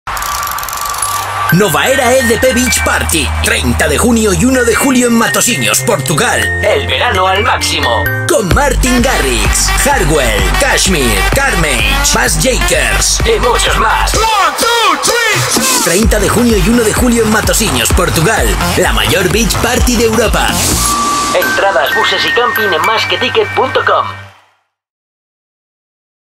EDP Beach Party
La Mayor Fiesta en la Playa de Europa

EDP Beach Party es una superproducción de la emisora NOVA ERA con el apoyo del Ayuntamiento de MATOSINHOS, que tendrá lugar los días 30 de Junio y 1 de Julio en la Playa de Aterro Norte, localidad de Matosinhos, Portugal.
Prepárate para disfrutar de la Mayor Pista de Baile en la Playa de Europa con el escenario más espectacular y los mejores DJs del mundo.
Se celebra la décima edición del exitoso festival portugués EDP Beach Party, que cuenta en su historial con nombres como Swedish House Mafia, Avicii, David Guetta, Axwell, Nervo, Steve Angello, Dimitri Vegas & Like Mike, Yellow Claw, Nicky Romero y Afrojack, que han brillado en sus anteriores ediciones. No cabe duda de que va a ser un evento sin igual al cual acudirá una gran cantidad de público.
El fin de semana de 30 de Junio y 1 de Julio te espera una gran fiesta plagada de música y grandes actuaciones en este espectacular festival,
 en un emplazamiento sin igual, en Playa de Aterro Norte, Matosinhos.

30 de Junio y 1 de Julio

Martin Garrix, Hardwell, KSHMR, Bassjackers, Carnage, Moksi, Matisse & Sadko,

Tony Junior, Mat Zo, Kris Kross Amsterdam, Dante Klein y muchos más!